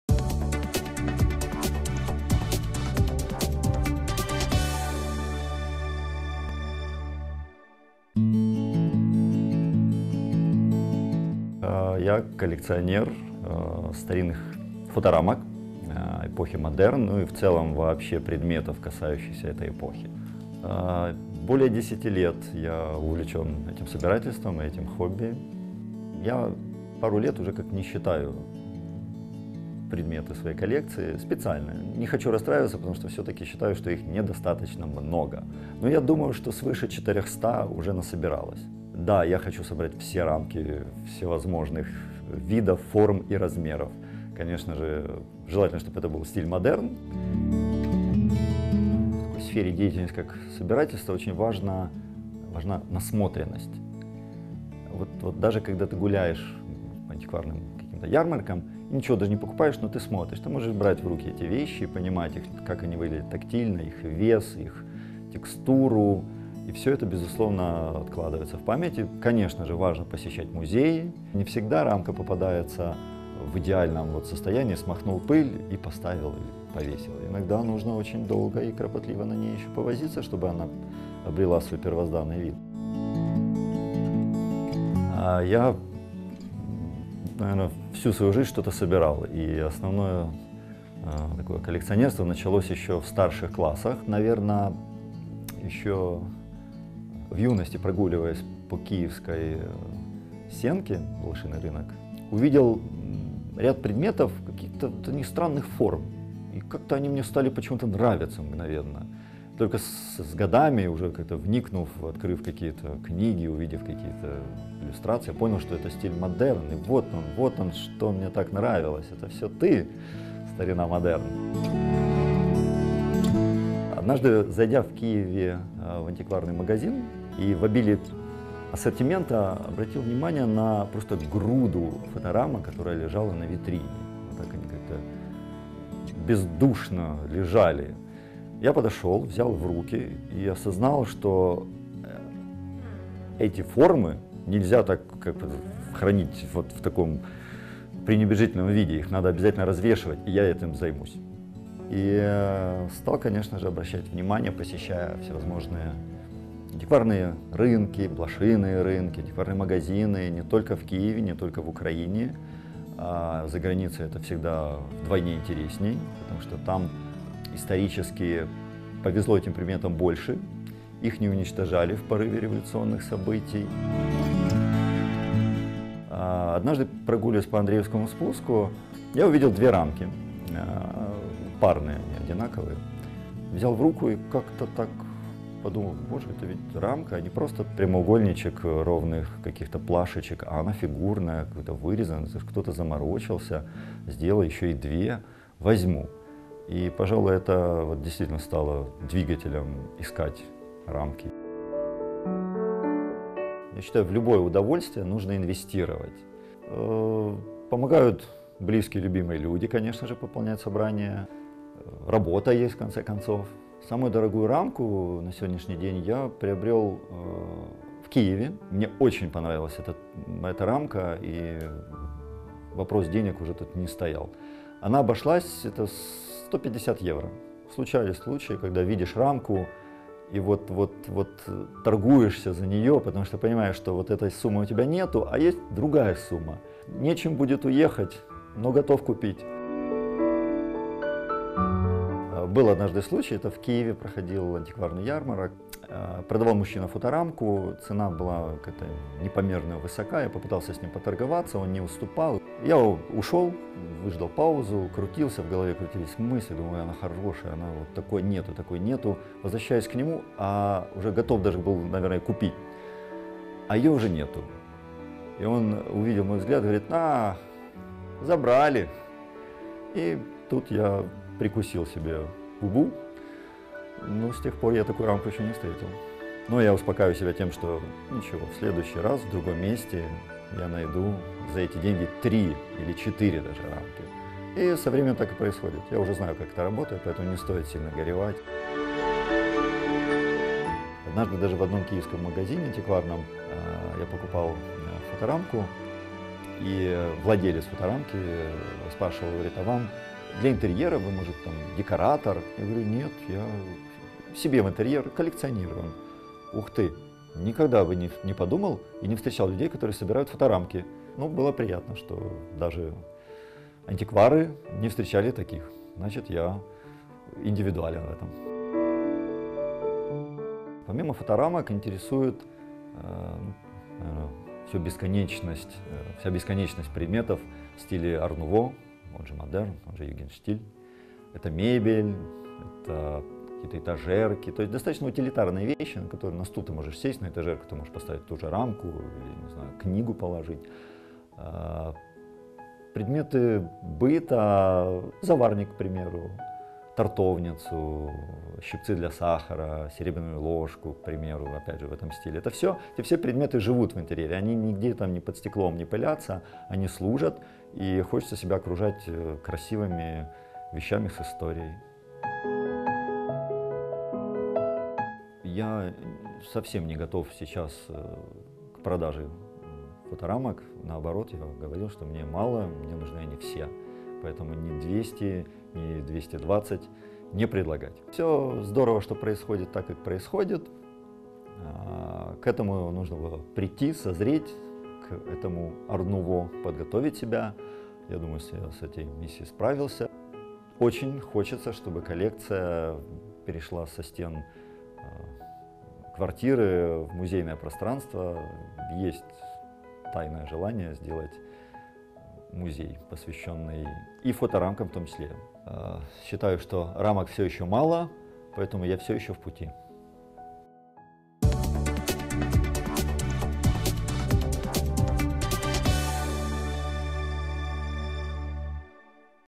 Павло Третьяков- колекціонер старовинних фоторамок
Павло Третьяков - київський колекціонер предметів побуту початку 20-го століття, виконаних в стилі модерн. Основу його колекції складають старовинні фоторамки родом з тієї епохи.